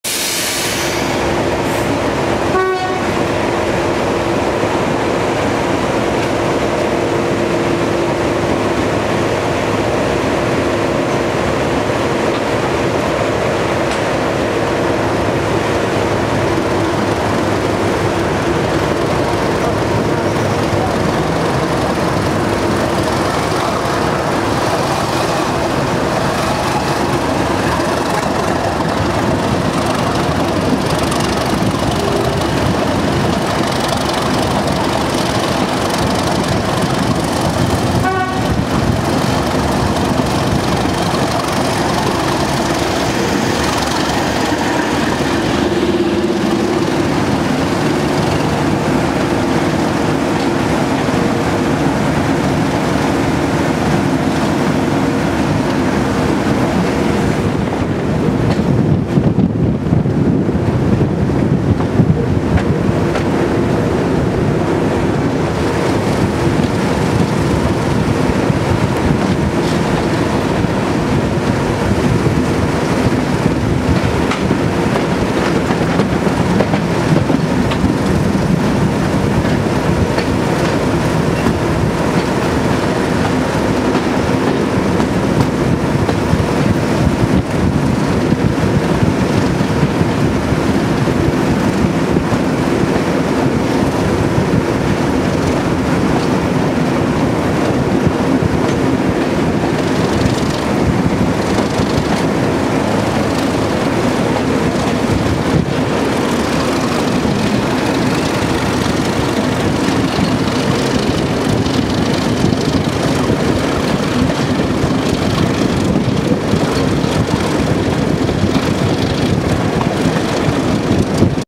Departing Groombridge with 37 254 and passing 31 206
We depart Groombridge at 13:57 heading to Eridge. As with the last video, Intercity liveried 37 254 is providing the power. Shortly after we depart Groombridge, we pass 31 206 in the loop with a service to Tunbridge Wells West.